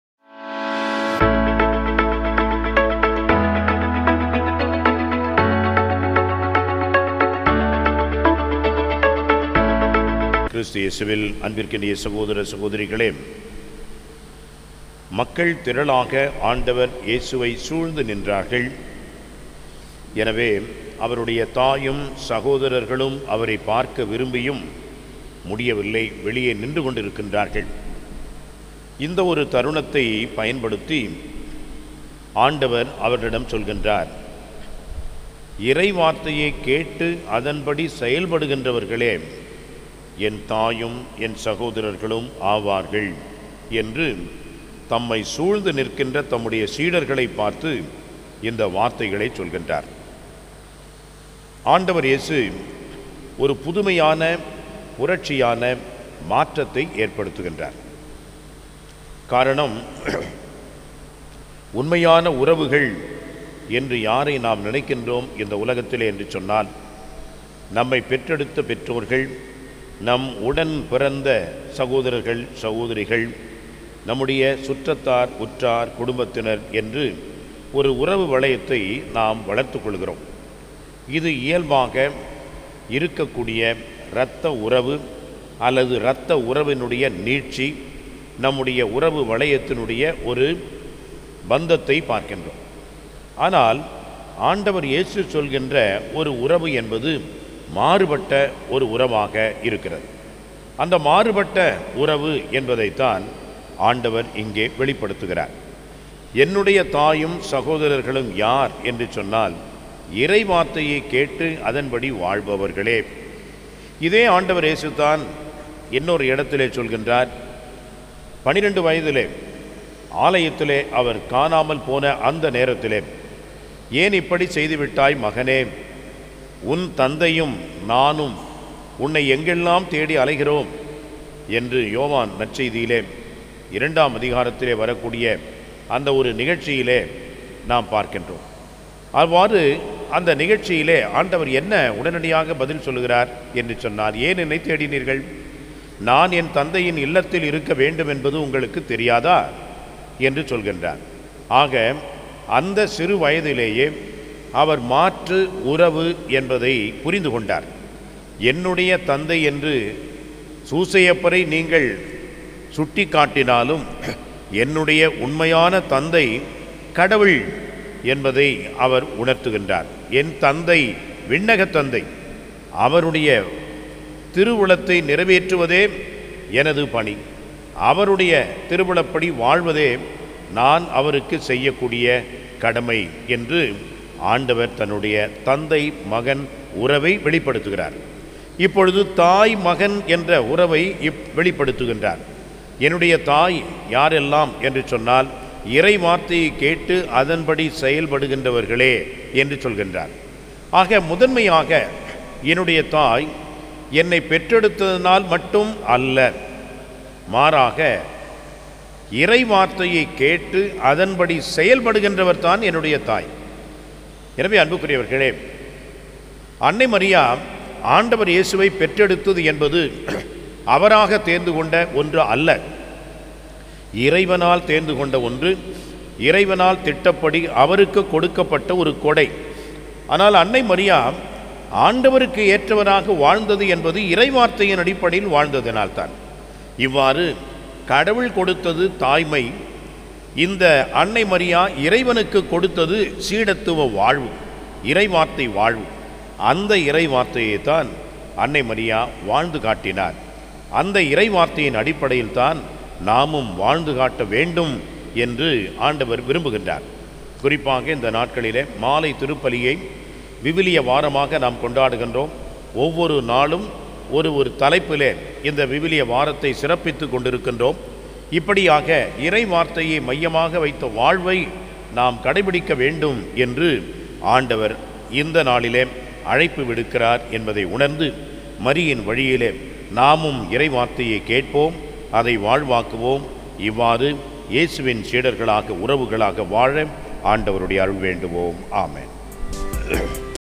என்னுடைய தாயும் என் சகோதரர்களும் யார்? மறையுரை சிந்தனைகள் RevFr S Pitchaimuthu Lourdes Tv 24.09.2024
Rev Fr S Pitchaimuthu, Rector & Parish Priest Sacred Heart Basilica Pondy